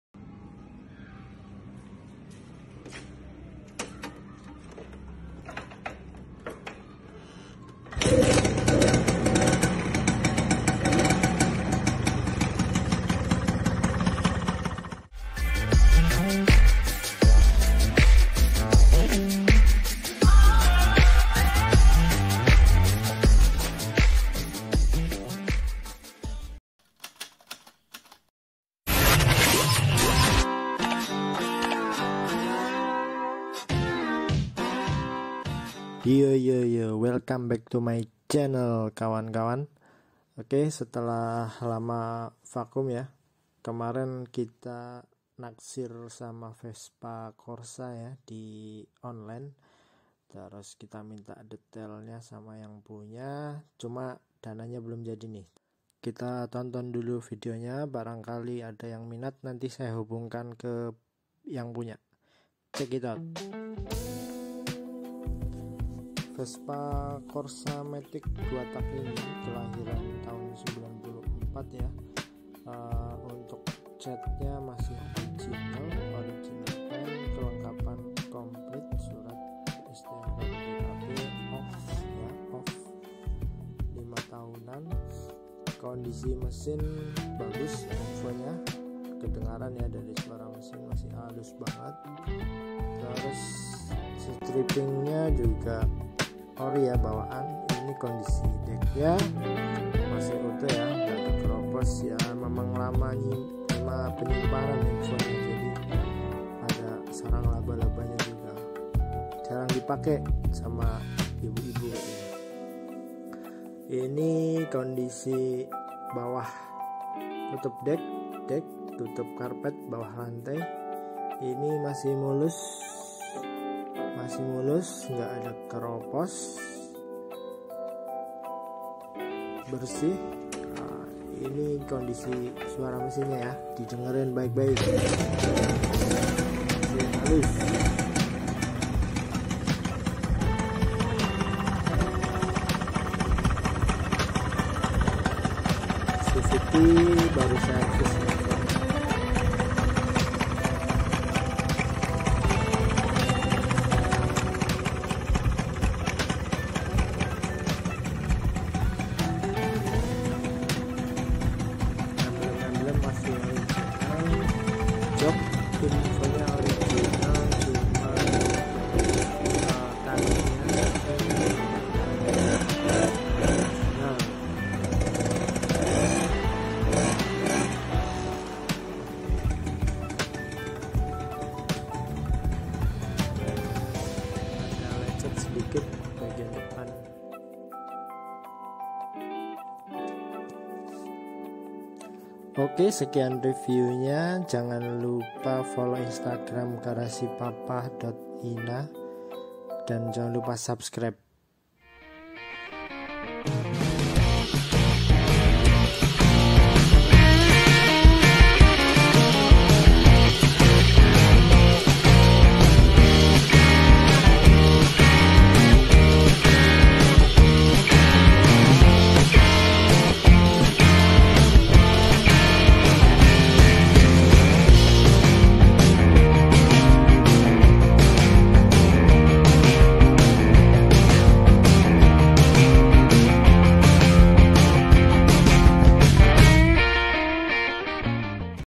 VESPA CORSA MATIC 2 TAK 1994
REVIEW VESPA CORSA 1994, Matic 2 tak idaman sayang dana JHT belum ada

Untuk jual beli vespa dan sparepart NOS murah yuk merapat dan follow instagram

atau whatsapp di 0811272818